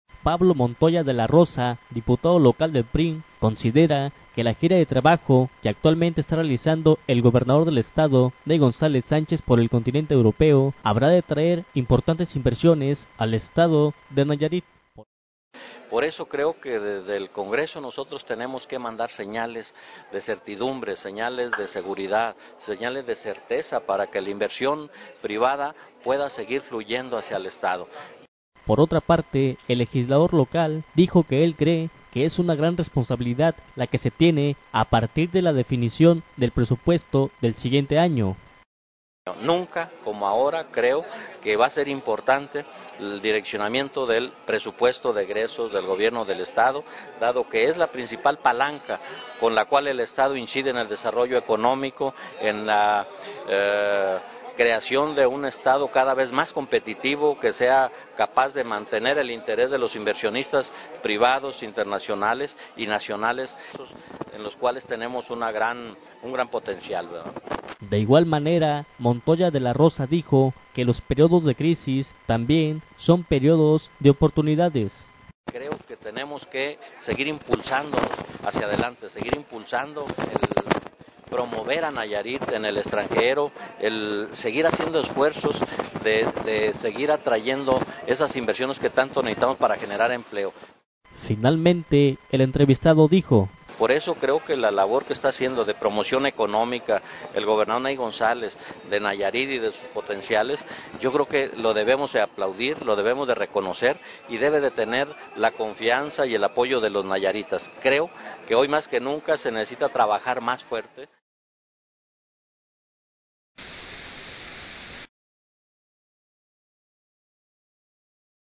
Nayarit La gira de trabajo de Ney en Europa traerá inversiones
Pablo Montoya de la Rosa, diputado local del PRI, considera que la gira de trabajo que actualmente está realizando el gobernador del estado, Ney González Sánchez, por el continente europeo, habrá de traer importantes inversiones al estado de Nayarit, por eso creo que desde el congreso nosotros tenemos que mandar señales de certidumbre, señales de seguridad, señales de certeza para que la inversión privada puede seguir fluyendo hacia el estado.
Por otra parte, el legislador local dijo que él cree que es una gran responsabilidad la que se tiene a partir de la definición del presupuesto del siguiente año: nunca como ahora, creo que va a ser importante el direccionamiento del presupuesto de egresos del gobierno del estado, que es la principal palanca con la cual el estado incide en el desarrollo económico, en la creación de un estado cada vez más competitivo, que sea capaz en mantener el interés de los  inversionistas privados internacionales y nacionales en la explotación de los recursos en los cuales tenemos un gran potencial.